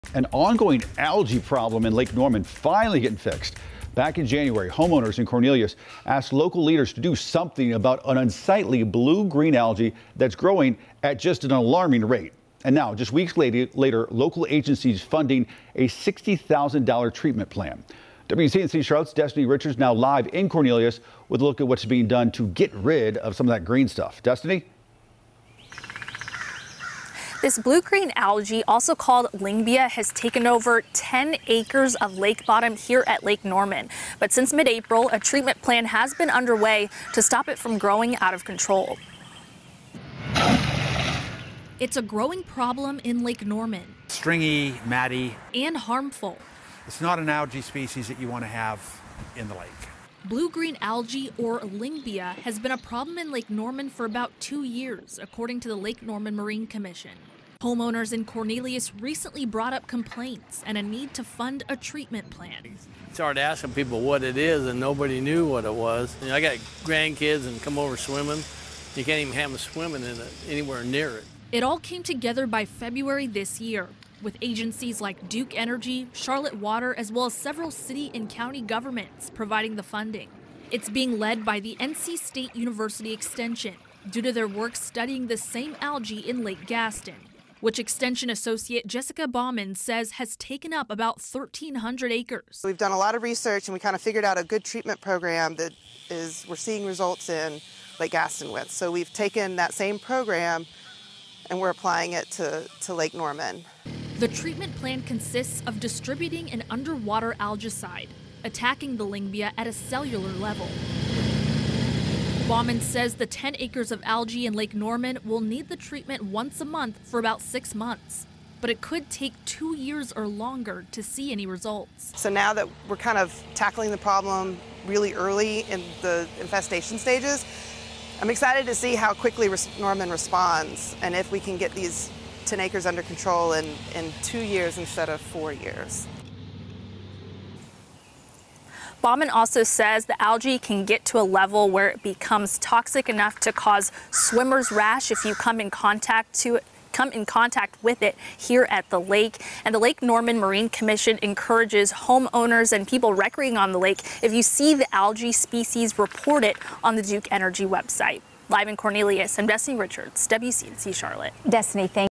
Lake Norman leaders tackling blue-green algae growth
It’s a growing problem in Lake Norman and it can become harmful: Blue-green algae.

But since mid-April, a treatment plan has been underway to stop it from growing out of control.

Bob Van Dolah, chairman of Lake Norman Marine Commission's Environmental Committee, said blue-green algae, or lyngbya, is an invasive algae that's been a problem for about two years.

"It’s not an algae species you want to have in the lake," Van Dolah said.

Early this year, homeowners in Cornelius brought up complaints and a need to fund a treatment plan.